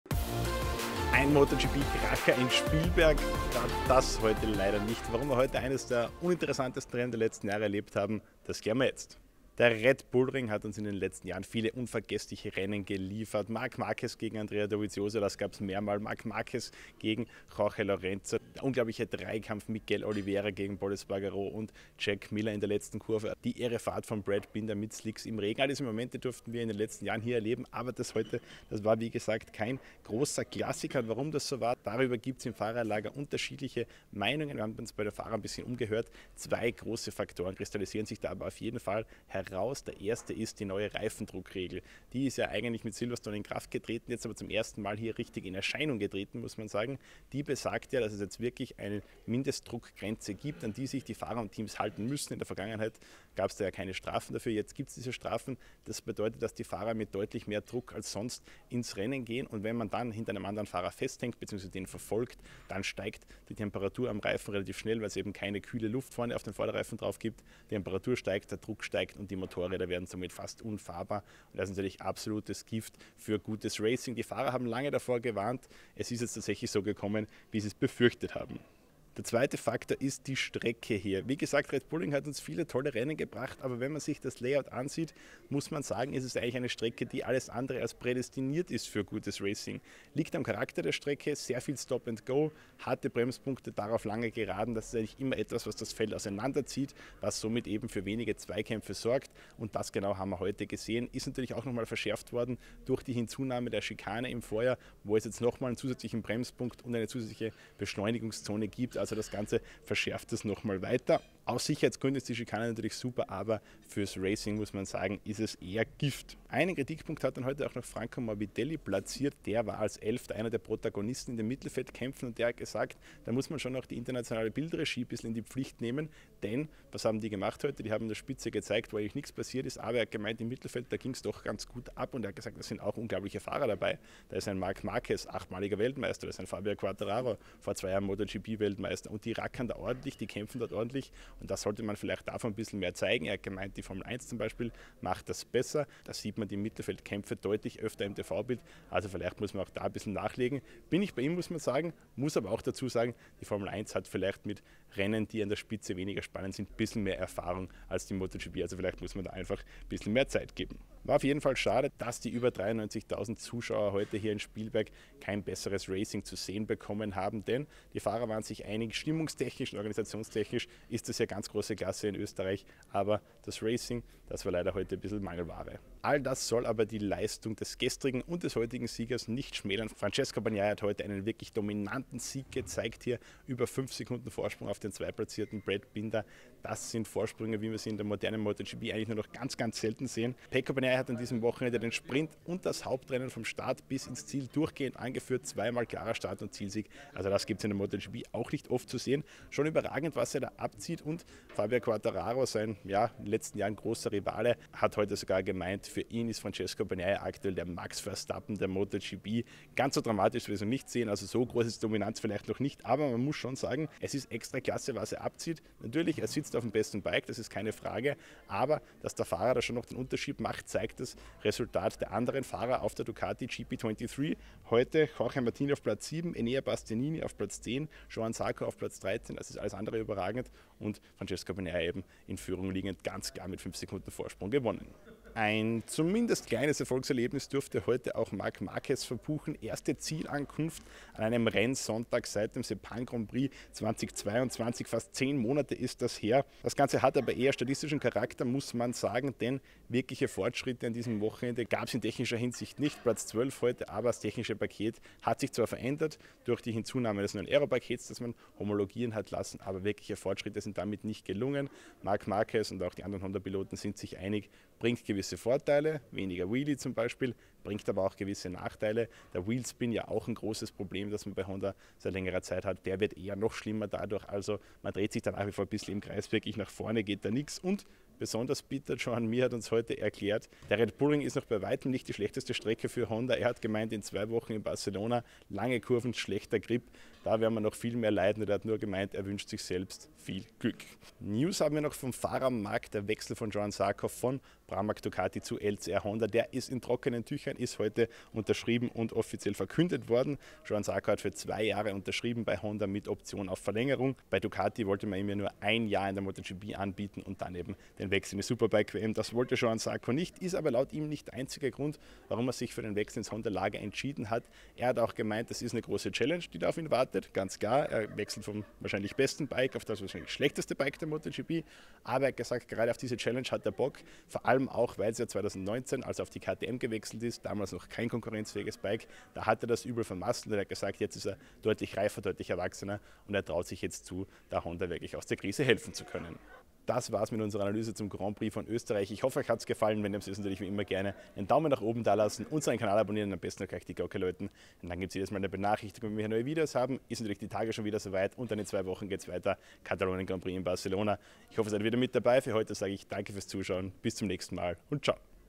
Warum herrschte MotoGP-Langeweile in Spielberg? Das ist der Racing-Killer!
Ein ganz großer MotoGP-Kracher war das am Sonntag nicht in Spielberg. Die Strecke, die uns in der Vergangenheit so viele überragende Rennen geliefert hatte, sorgte für eine maue Show. Über die Gründe dafür gibt es unterschiedliche Meinungen, zwei Faktoren kristallisierten sich aber im Gespräch mit den Fahrern heraus. Zum einen: Die neue Reifendruckregel, die ein dichtes Verfolgen anderer Piloten fast unmöglich macht. Zum anderen: Das Layout am Red Bull Ring. Auch Kritik an der TV-Regie blieb nicht aus. All das soll aber nicht die Leistung von Francesco Bagnaia schmälern, der in Spielberg sowohl im Sprint als auch im Grand Prix jeden einzelnen Meter führte und die Konkurrenz in Grund und Boden fuhr.

Wir suchen Verstärkung! Bewirb dich jetzt bei Motorsport-Magazin

Übersicht:
00:00 Intro & Themen
00:11 Racing: Gründe für die Überholflaute
02:54 Bagnaia: Dominanz in Spielberg
04:03 M. Marquez: Erste Sonntagspunkte
04:50 Fahrermarkt: Zarco-Wechsel fix